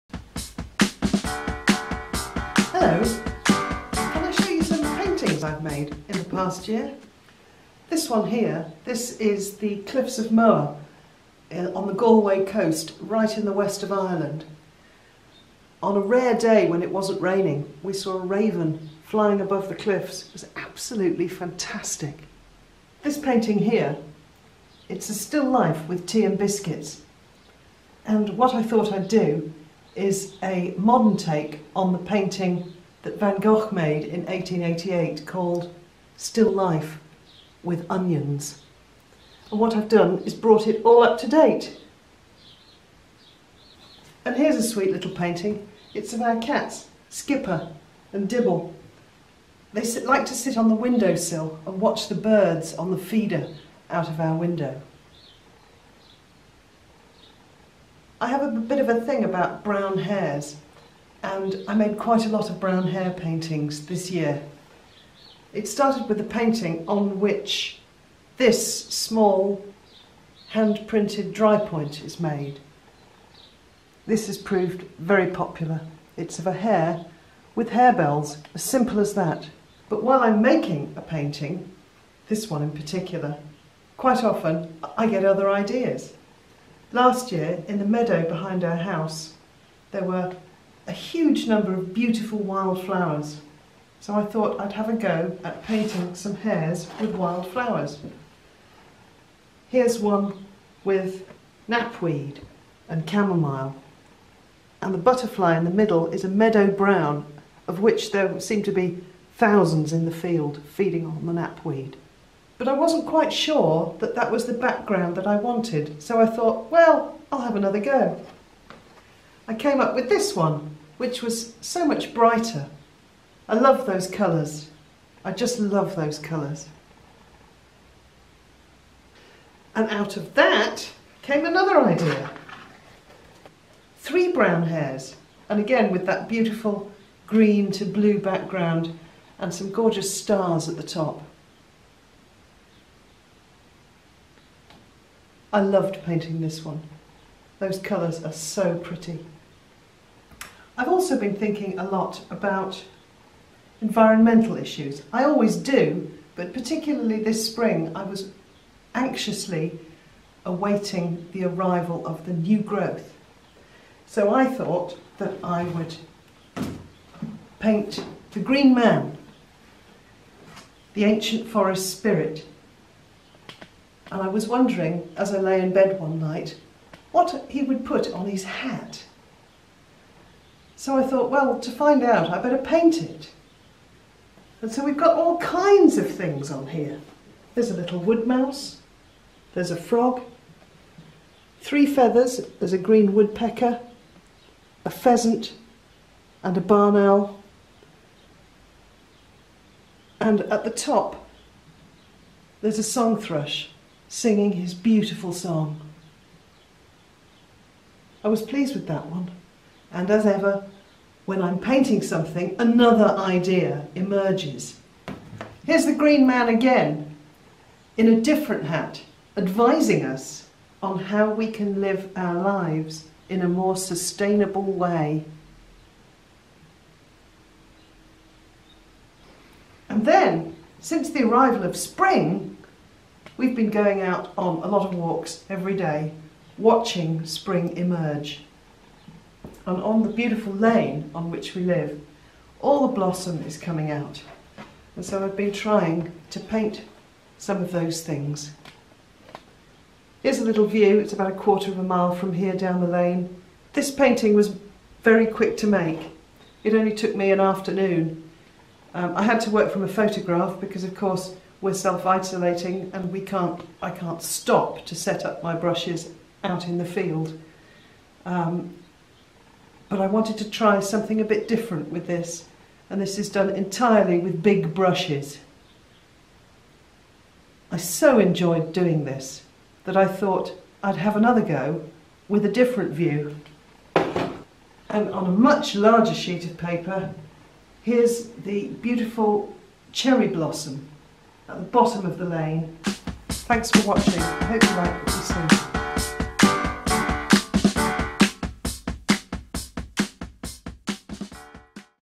Recent paintings by Jane Tomlinson
Artist Jane Tomlinson shows some of her recent paintings and explains how she came to paint them. Jane invites you to her online exhibition.